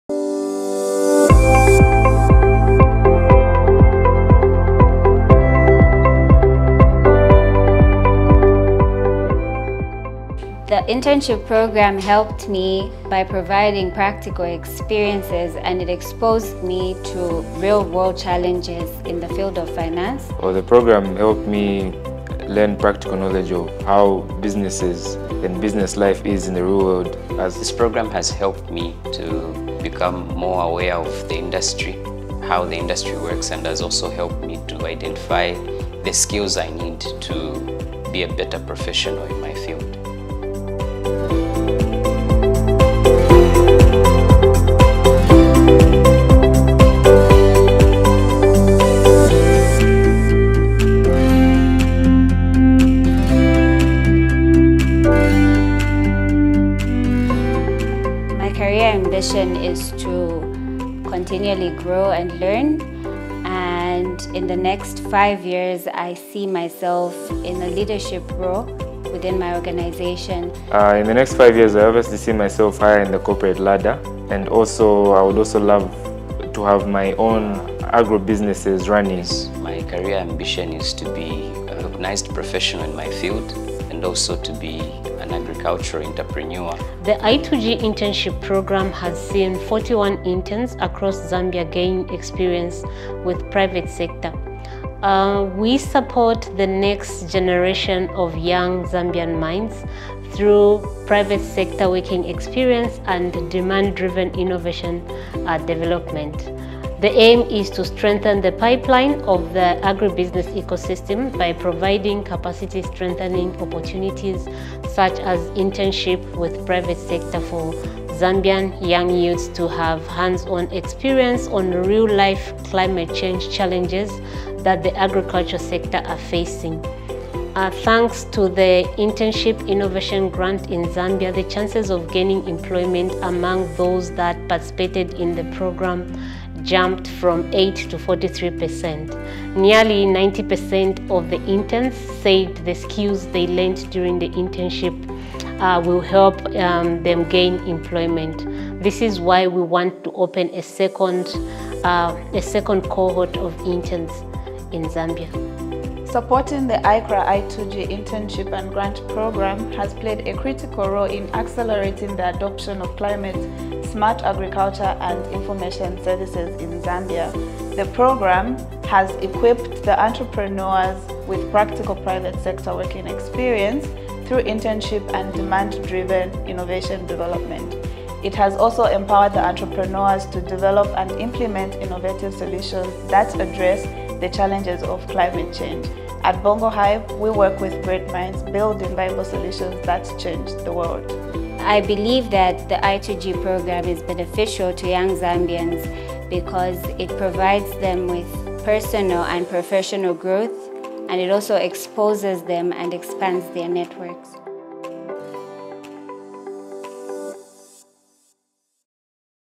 AICCRA Zambia: i2G connects youth and the private sector for climate-smart agriculture
The aim of the Internship and Innovation Grant (i2G) is to strengthen the pipeline of the agribusiness ecosystem by providing capacity strengthening opportunities for Zambian youth to tackle real-life climate-smart agriculture (CSA) and climate information services (CIS) challenges faced by the agriculture sector.

To date, the i2G program has seen 41 young innovators, entrepreneurs, professionals and graduate students from across Zambia receiving hand-on experience in the challenges and solutions in the country’s agricultural sector.

In collaboration with Zambia Enterprise Development and Growth Enhanced (EDGE), Agricultural Cooperative Development International/Volunteers in Overseas Cooperative Assistance (ACDI/VOCA) and Zambia’s first innovation and technology hub called BongoHive.